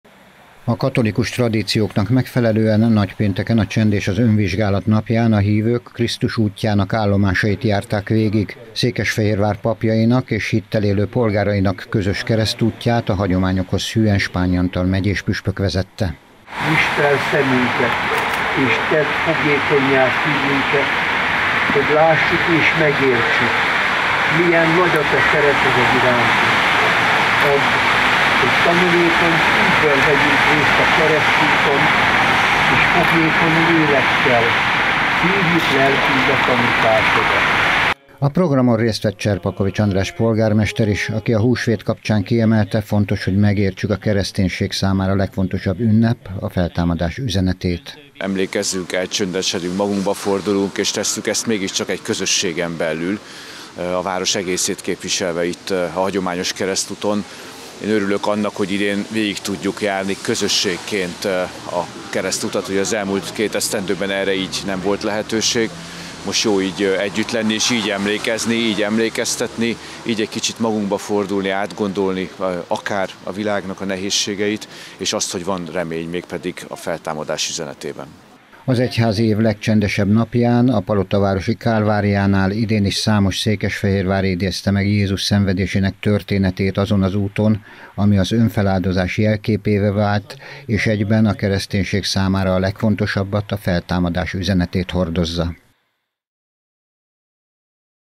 KERESZTÚT A PALOTAVÁROSBAN
Székesfehérváron hagyomány, hogy a város papjai és lakói közösen emlékeznek a Palotavárosi kálváriánál Jézus kereszthalálára, arra az útra, ami az önfeláldozás jelképévé vált. A keresztutat Spányi Antal megyés püspök vezette.